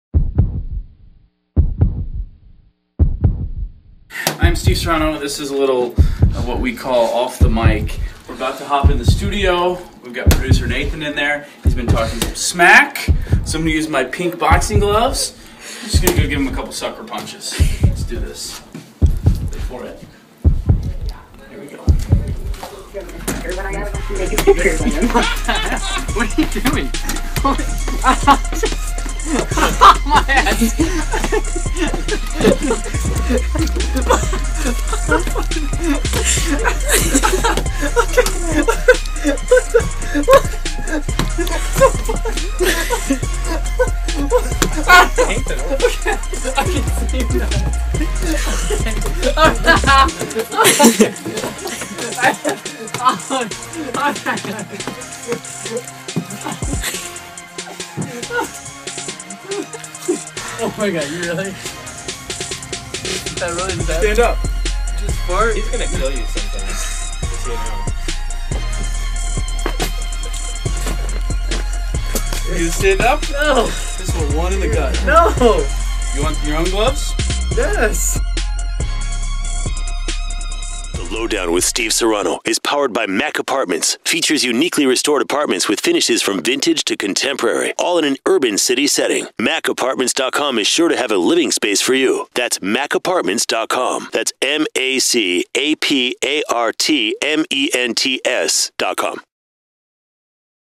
Off the Mic
Steve Serrano takes down Producer Nathan.
Its about to get real.